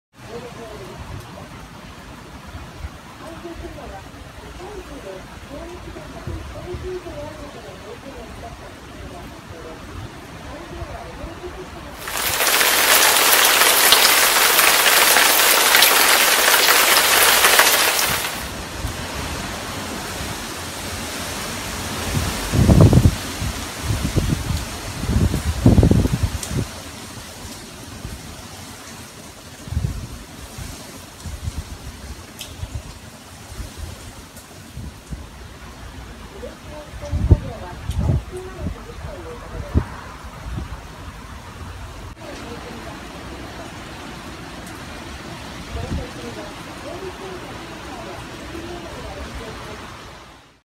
More Wind & Rain in Kobe!
Severe Tropical Storm 5 ‘Meliksi - downgraded from a Typhoon - causes more wind and rain in Kobe.